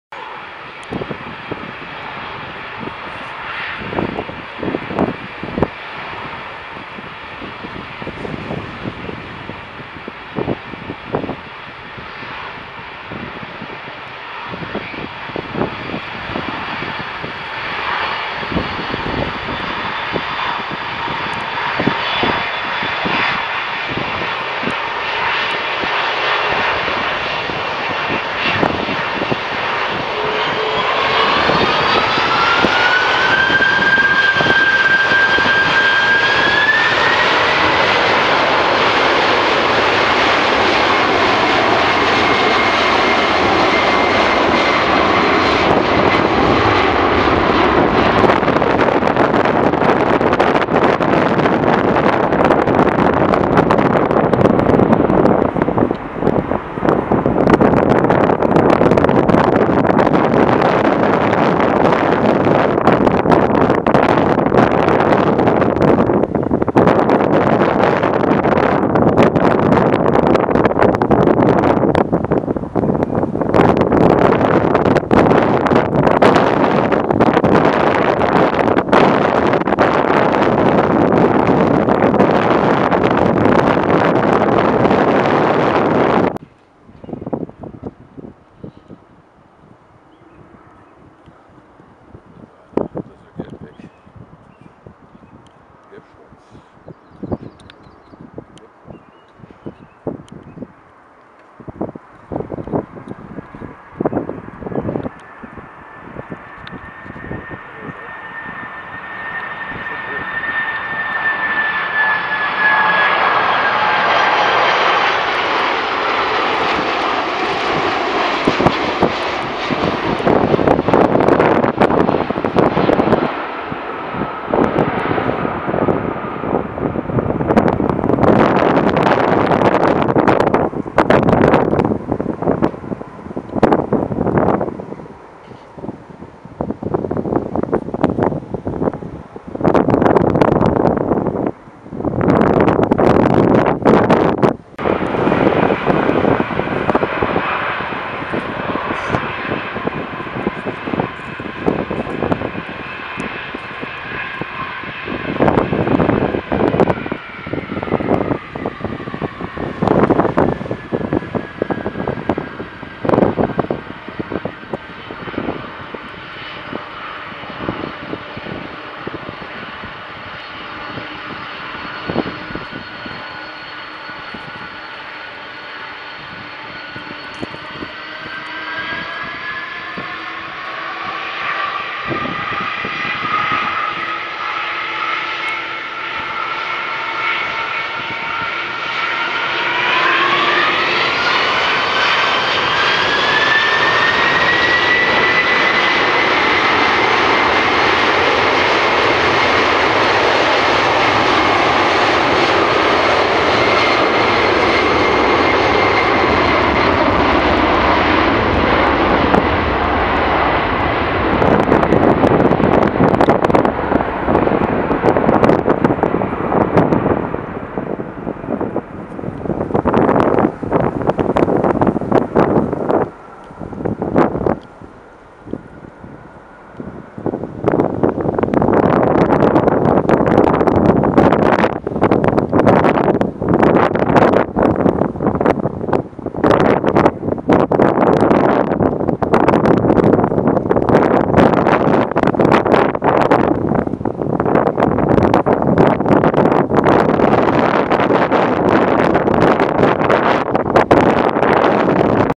Heavy Cargo Action @ PANC - Boeing 747-400F
UPS (United Parcel Service) Boeing 747-400F [N581UP] CN: 25866
Delivered to Cargolux on 17 November 1993 as LX-FCV
Transferred to UPS: 1 September 2009
Flight info: UPS59 to Louisville Int'l (KSDF) Duration: 5 hours 50 minutes @ FL330

Polar Air Cargo Boeing 747-400F [N453PA] CN: 30811
Delivered: 26 September 2001
Flight info: PAC237 from Northern Kentucky Int'l (KCVG) Duration: 6 hours 13 minutes @ FL360

Great Wall Airlines (China Cargo Airlines) Boeing 747-400F [B-2433] CN: 28027
Delivered to Singapore Airlines Cargo as 9V-SFI on 28 September 2000
Transferred to China Cargo Airlines: 1 June 2011
Flight info: CKK228 to Shanghai Pudong Int'l (ZSPD/PVG) Duration: 8 hours 25 minutes @ FL330